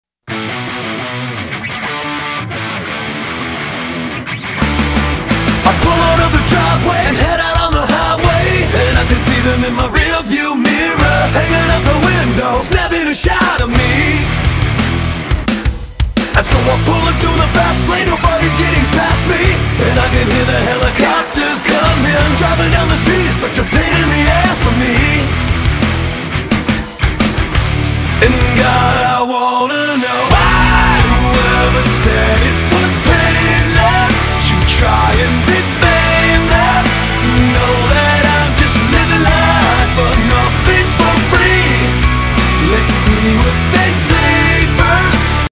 Valkyrie on US 82
Honda Valkyrie passing a truck and then being passed by a Suzuki Boulevard M109.  US HWY 82 between Ringgold and Nocona Texas.